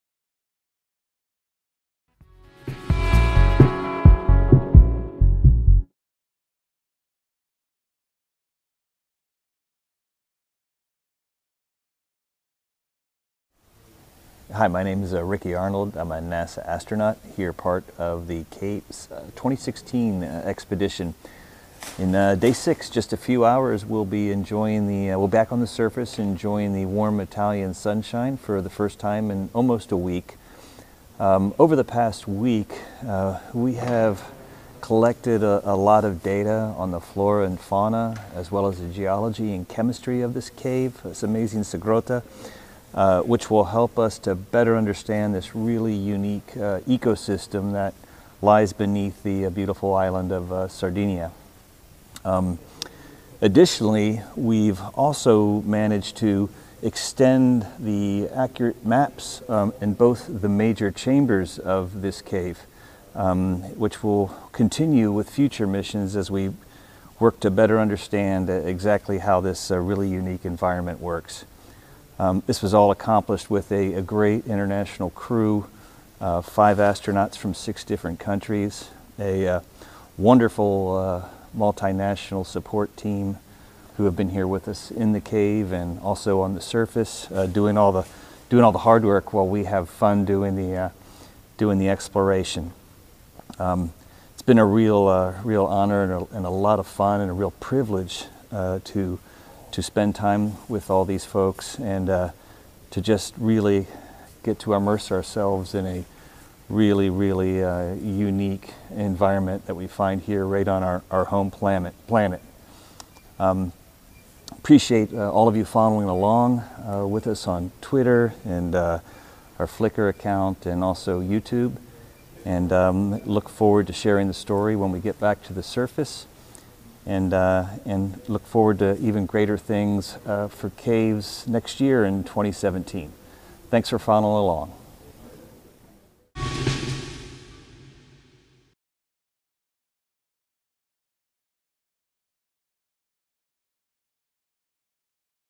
CAVES 2016: Video log day 6
NASA astronaut Ricky Arnold’s video log of the sixth and last day of the extended exploration phase of ESA’s underground training course CAVES.

Six astronauts from China, Japan, USA, Spain and Russia will spend six nights in caves of Sardinia, Italy, to explore the depths and train for life in outer space.

Ricky ended the course as commander after taking over from Japanese astronaut Aki Hoshide halfway through the mission. In this video Ricky discusses the exploration with the international crew.

CAVES – Cooperative Adventure for Valuing and Exercising human behaviour and performance Skills – focuses on multicultural approaches to leadership, following orders, teamwork and decision-making. At the end of the course the astronauts will have a better understanding of how they function in a multicultural team and what they are skilled at, as well as areas for self-improvement.

Follow the underground adventure on Twitter with via @ESA_CAVES, and on the CAVES blog